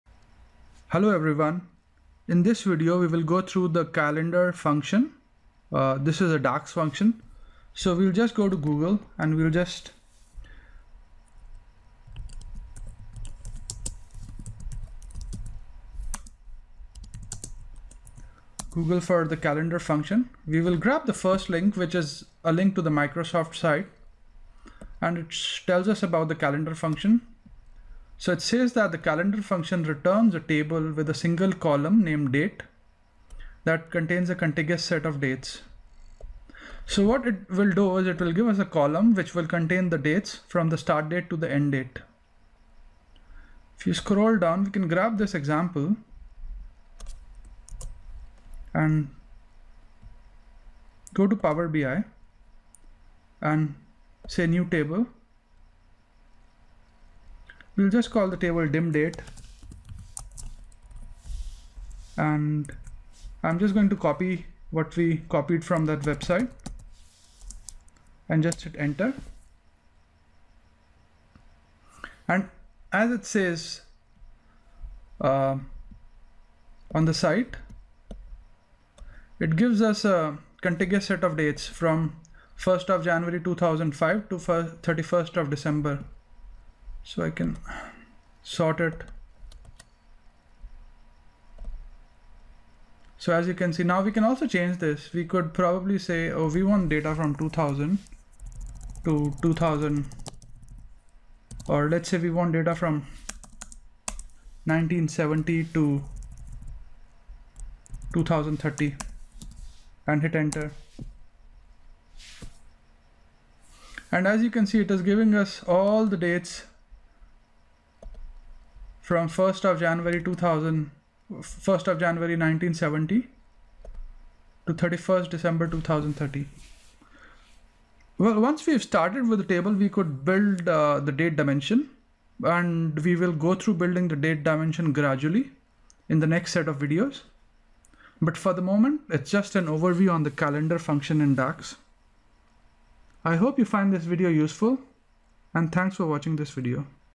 DAX Calendar Function Power BI
Using the DAX calendar Function to build a date liist in power BI